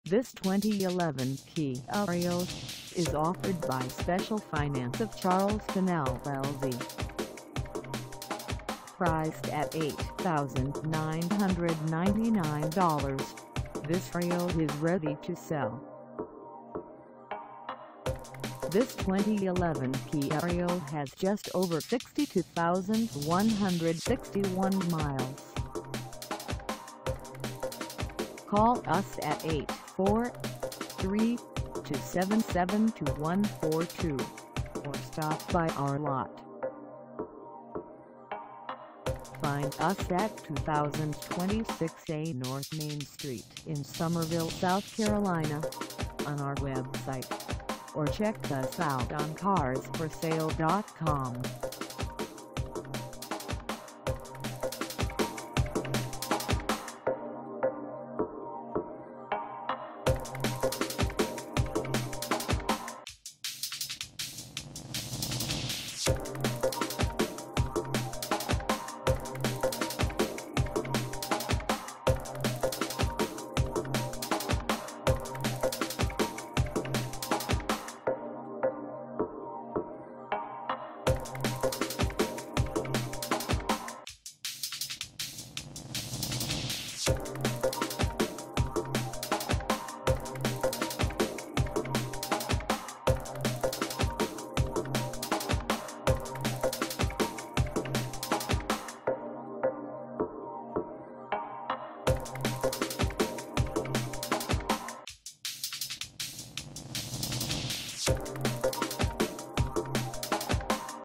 2011 Kia Rio LX 4dr Sedan for sale in Summerville, SC 29483
This 2011 Kia Rio LX 4dr Sedan is for sale in Summerville, SC 29483 at Special Finance of Charleston LLC.
Engine: I4 1.6L I4
Transmission: Automatic 4-Speed
Color: DK GRAY
Mileage: 62161
Contact Special Finance of Charleston LLC 2026-A North Main Street Summerville, SC 29483 to test drive this 2011 Kia Rio LX 4dr Sedan today.